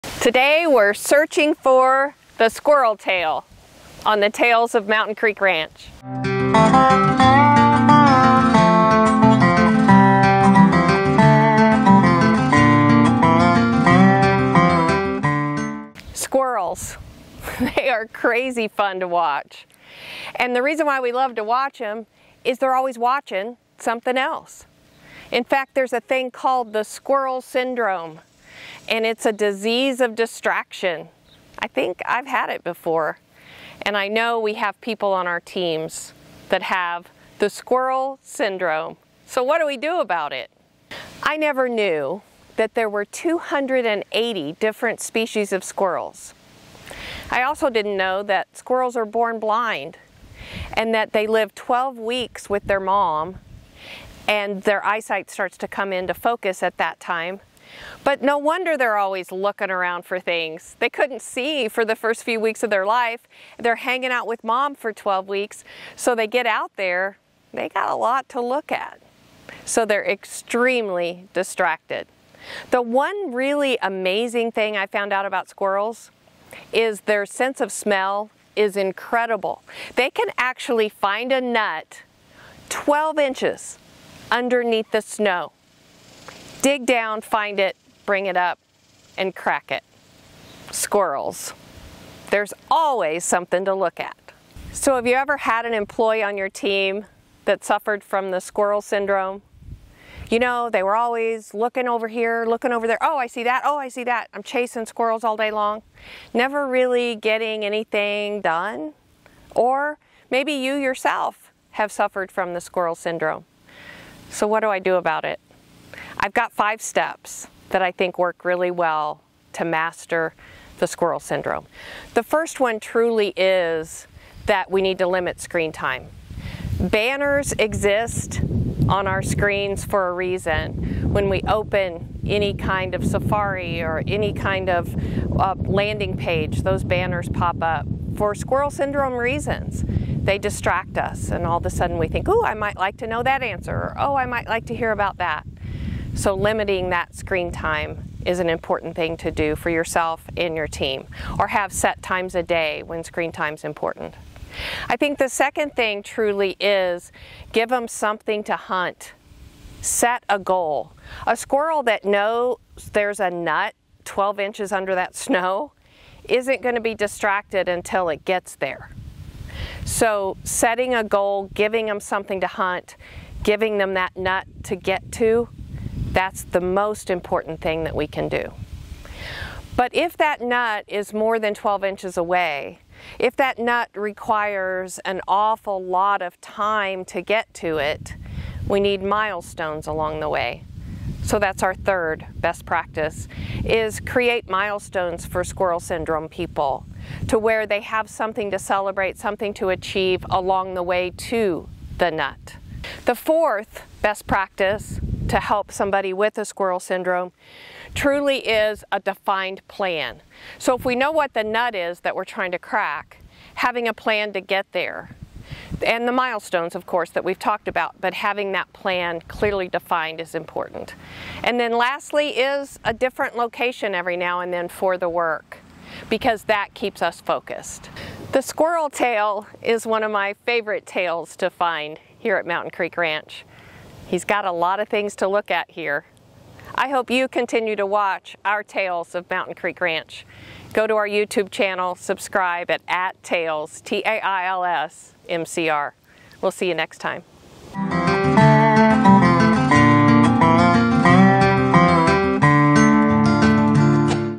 Tails 43: Squirrel Syndrome
Have you ever had someone on your team that suffered from “squirrel syndrome”…never really getting anything done? In this episode of Tails, Terri talks through 5 steps that can help you or a team member who suffers from squirrel syndrome.